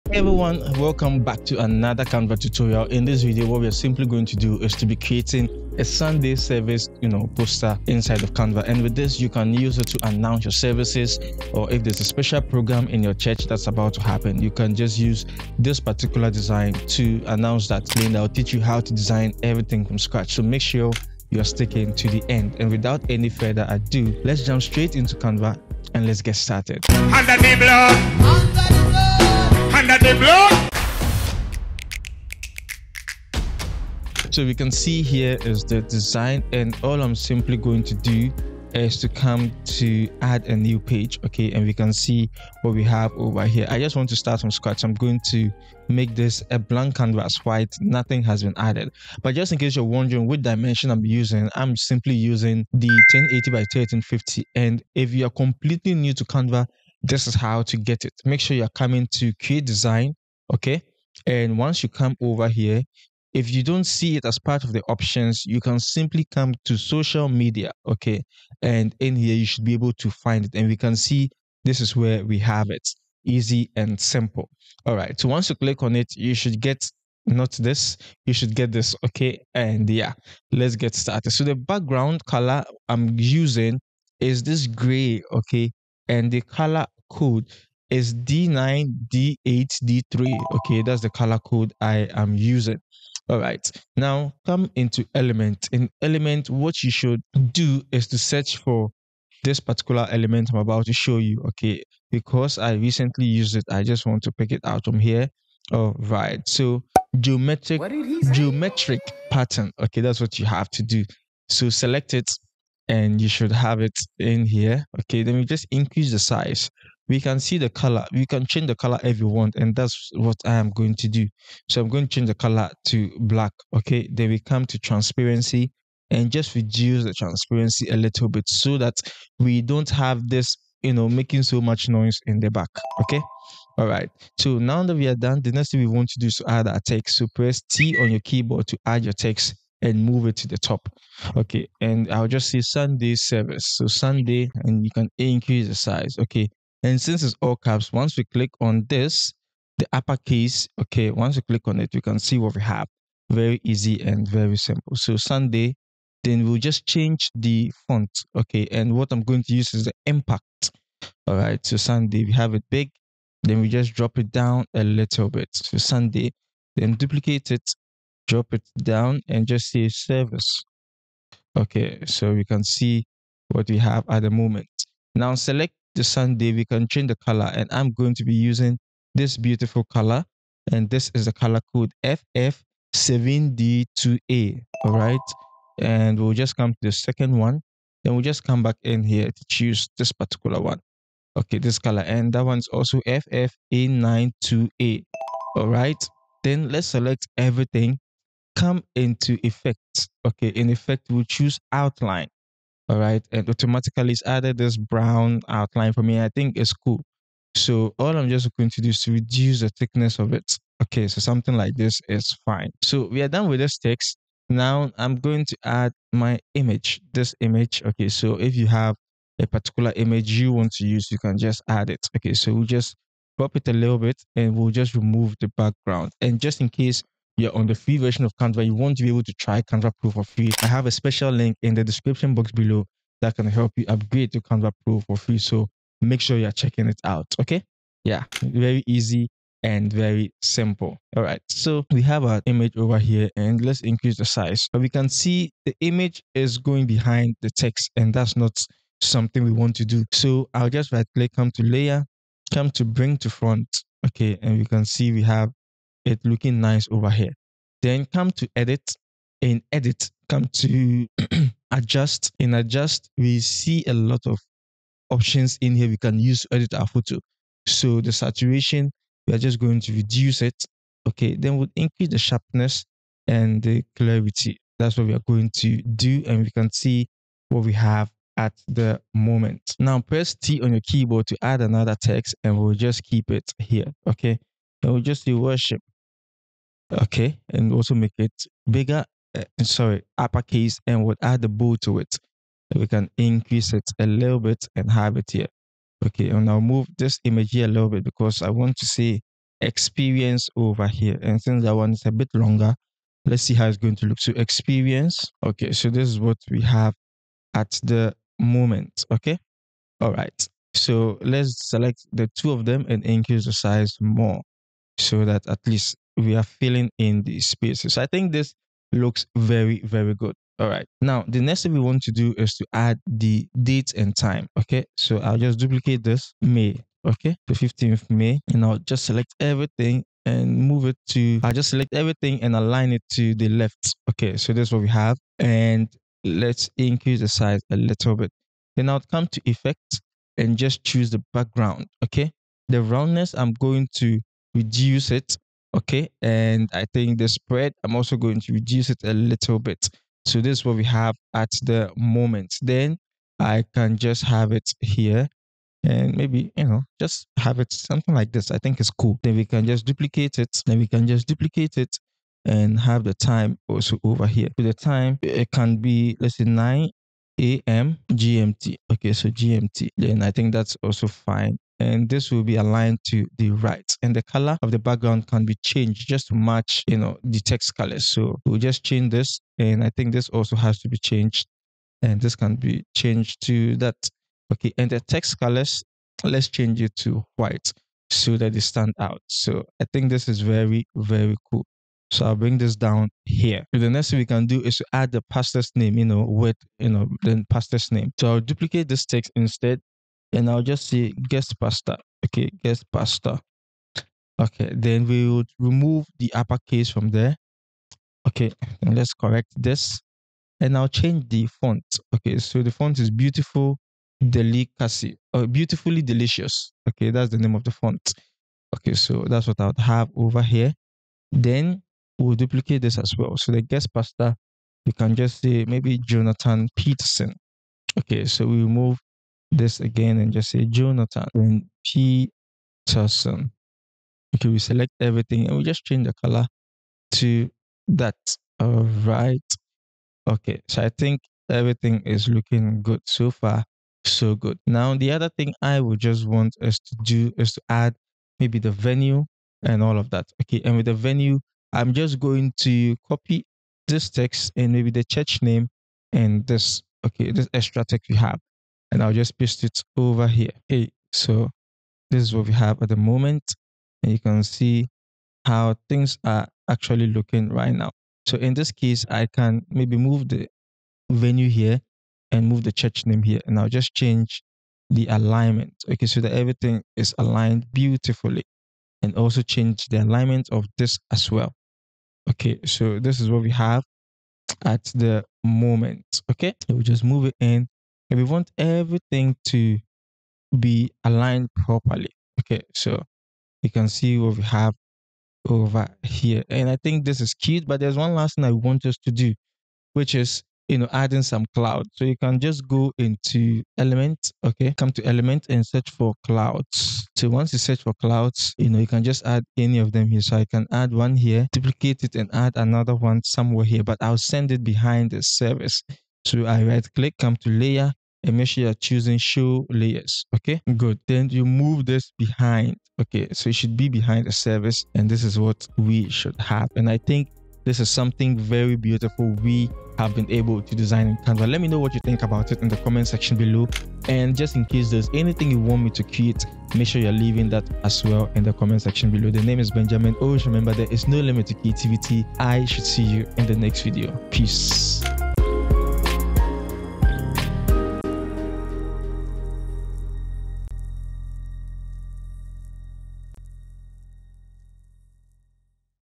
Design Your Own Church Poster Step by Step in Canva!
Want to create a beautiful and impactful church poster without any design experience? In this easy Canva tutorial, I’ll walk you through how to design a church event poster step-by-step, using free tools in Canva. Perfect for Sunday services, youth events, or special programs!